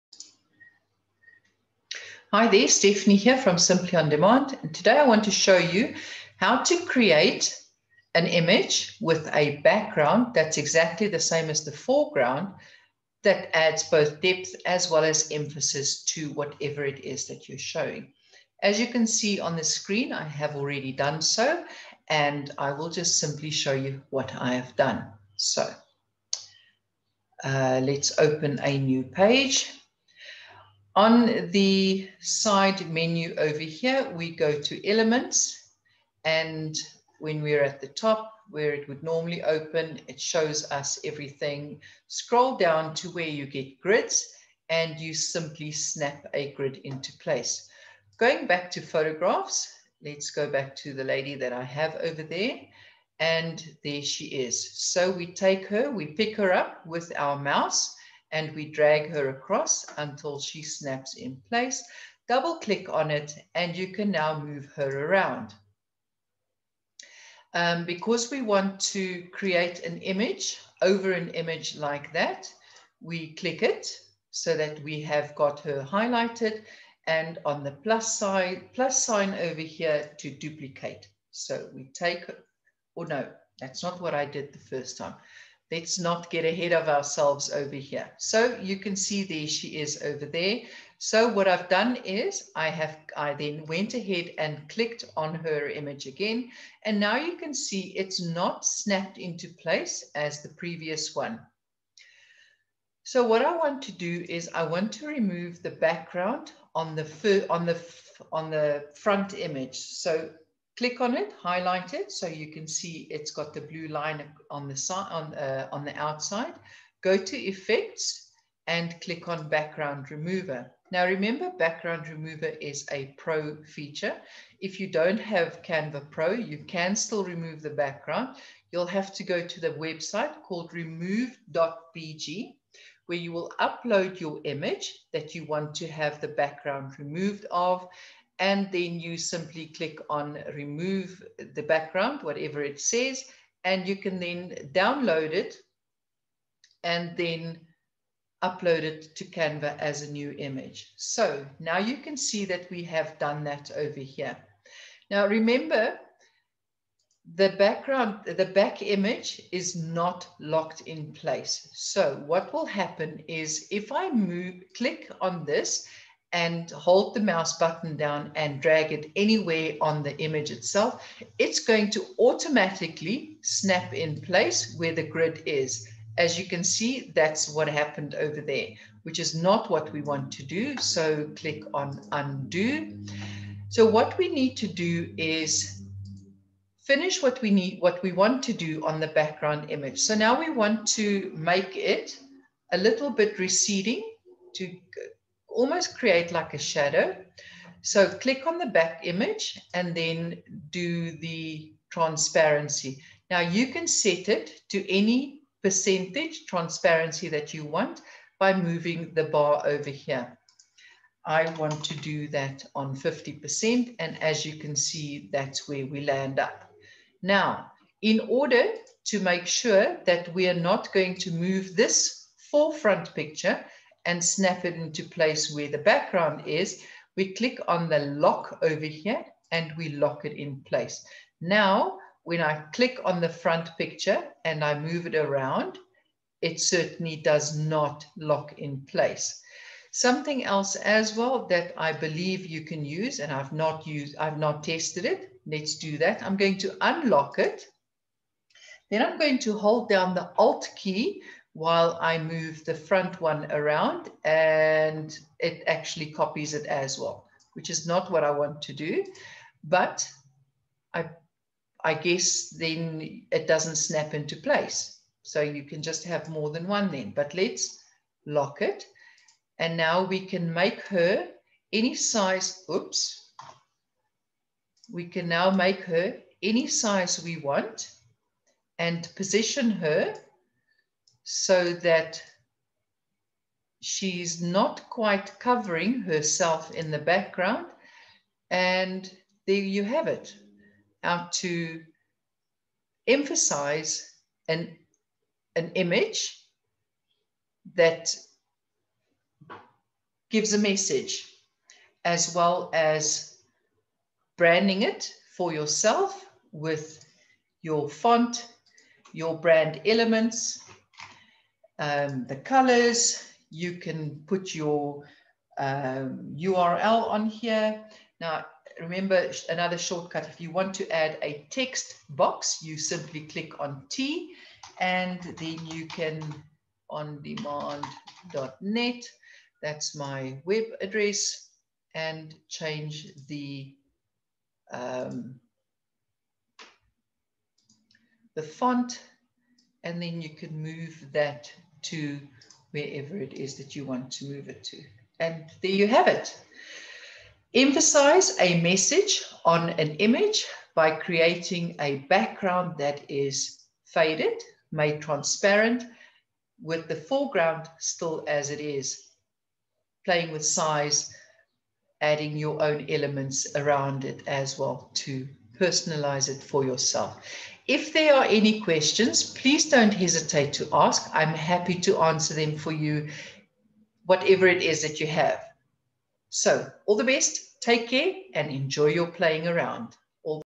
Adding depth & emphasis to an image in Canva
There comes a time when you want to play around with effects, especially depth, as it adds just that little "extra" to the design itself.

This video shows you how to do just that.